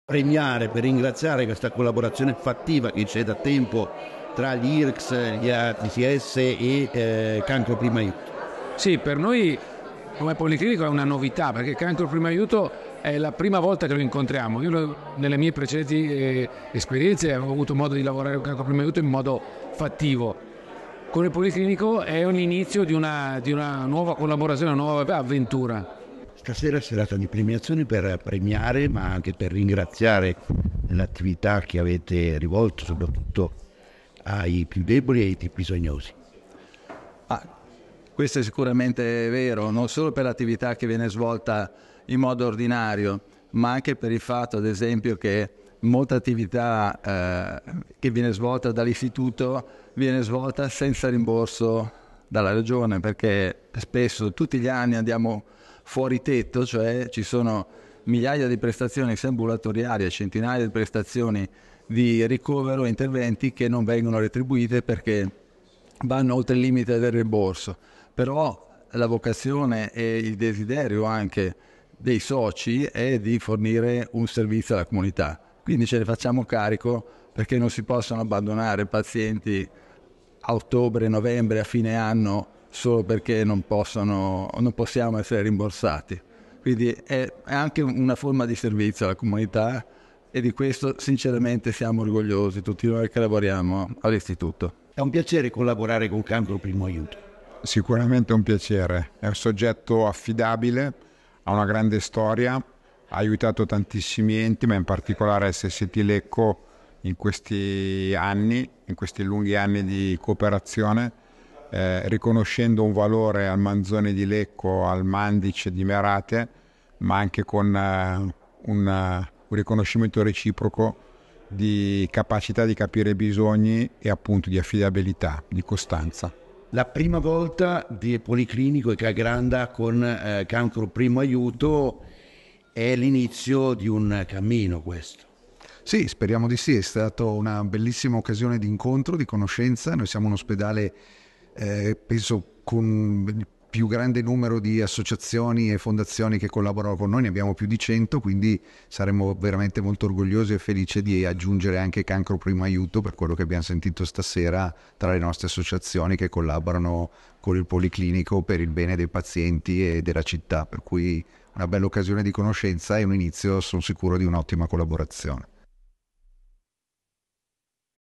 SANITA': FARE SQUADRA PER FARE MEGLIO
In Villa Walter Fontana a Capriano di Briosco, sede istituzionale di Cancro Primo Aiuto, una serata per sancire future collaborazioni tra la nostra Associazione e il Policlinico di Milano, gli Ospedali di Lecco e Merate e l'Istituto Europeo di Oncologia di Milano IEO @cancroprimoaiuto_